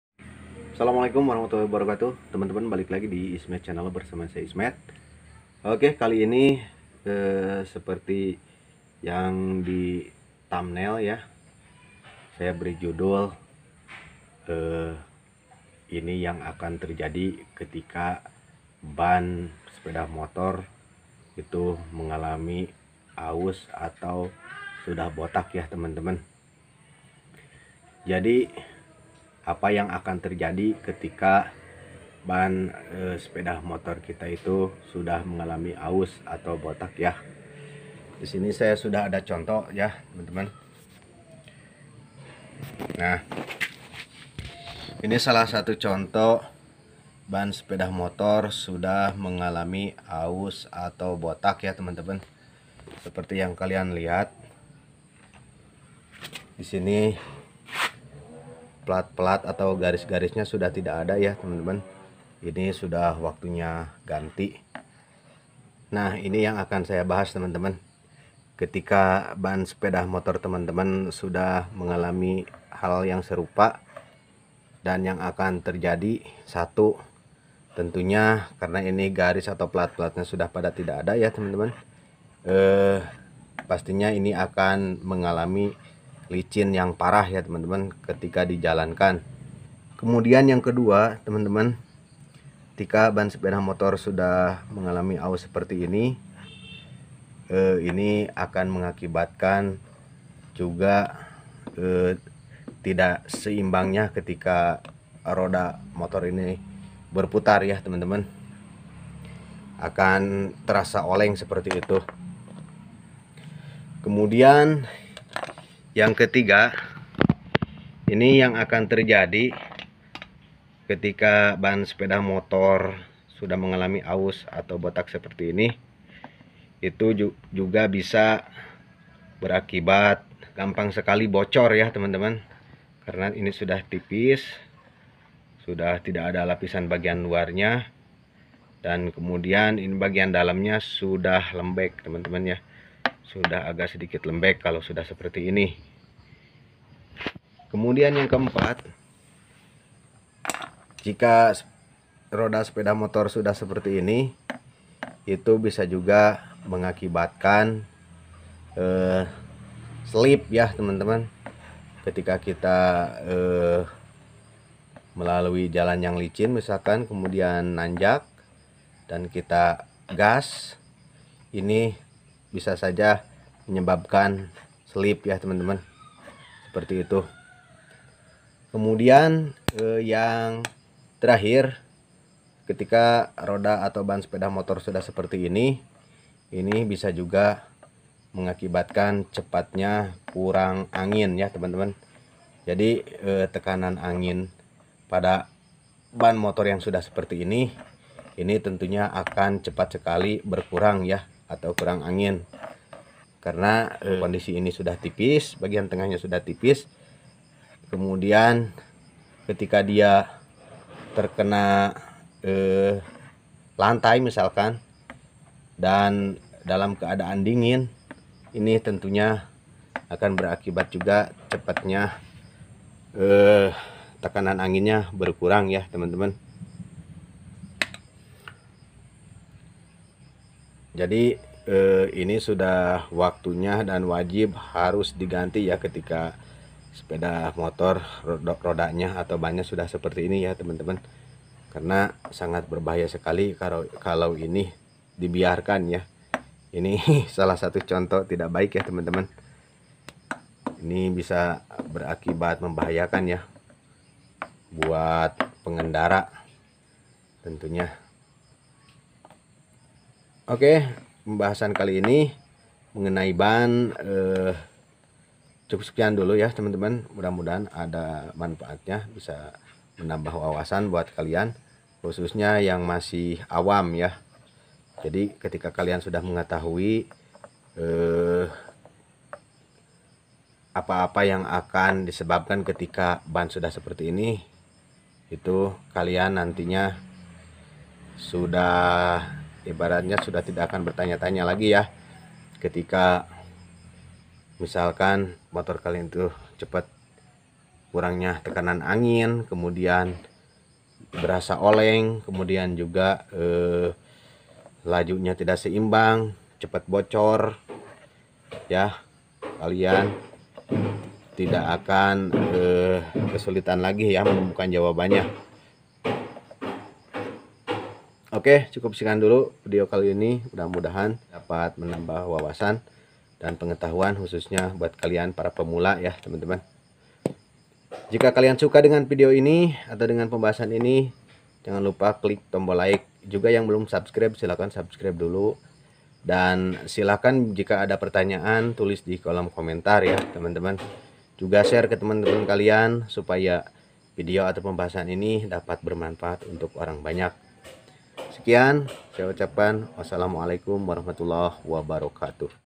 Akibat ban botak/aus||ini yang akan terjadi
Akibat ban motor sudah mengelupas :
1.mudah slip.
2.rawan bocor.
3.tidak seimbang ketika berputar.
4.tekanan angin mudah berkurang.
5.licin.
        Jika ada pertanyaan, silahkan tulis di kolom komentar.terimakasih